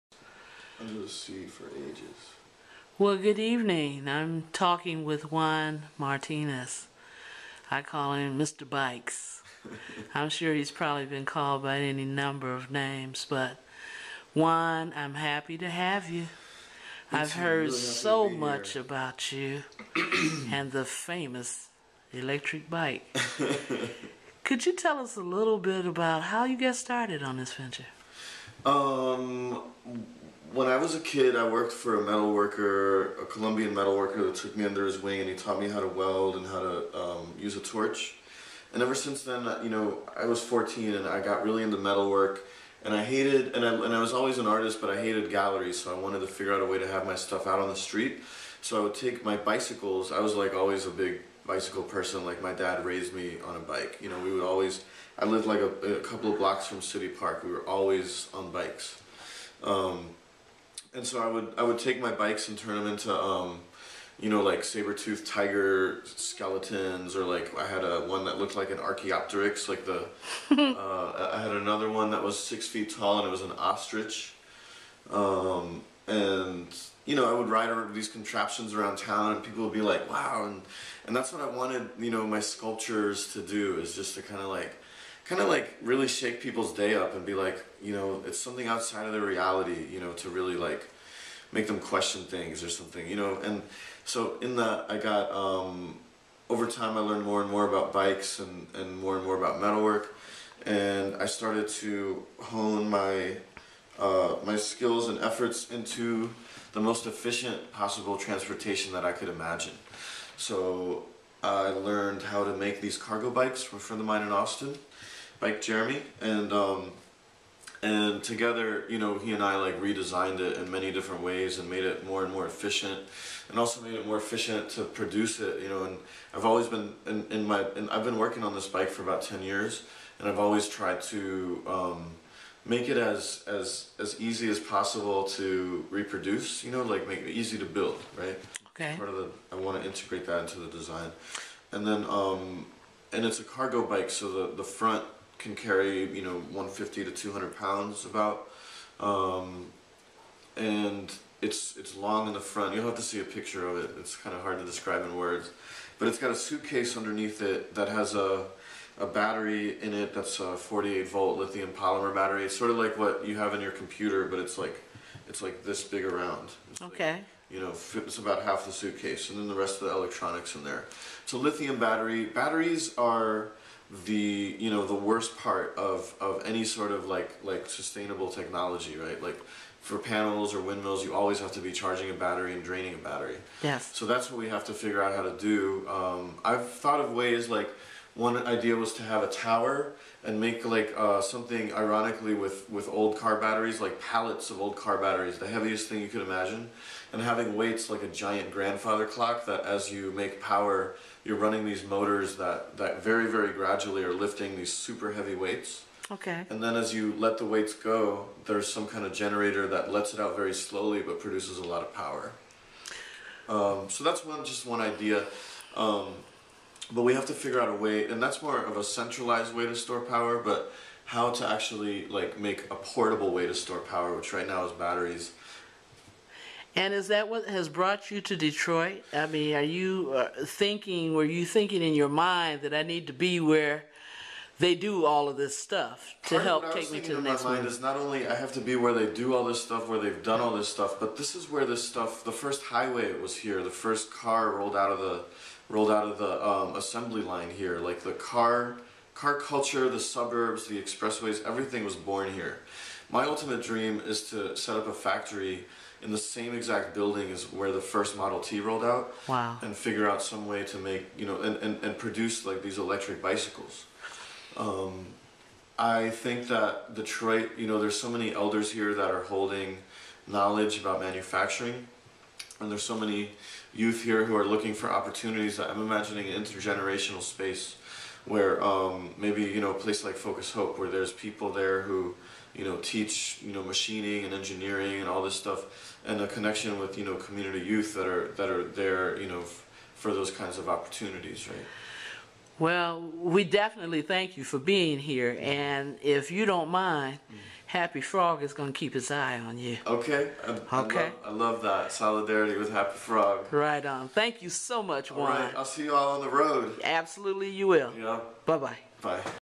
Juan Martinez, The Man About Town
Recorded on January 10, 2012  at the Project House in Detroit, on the East Side.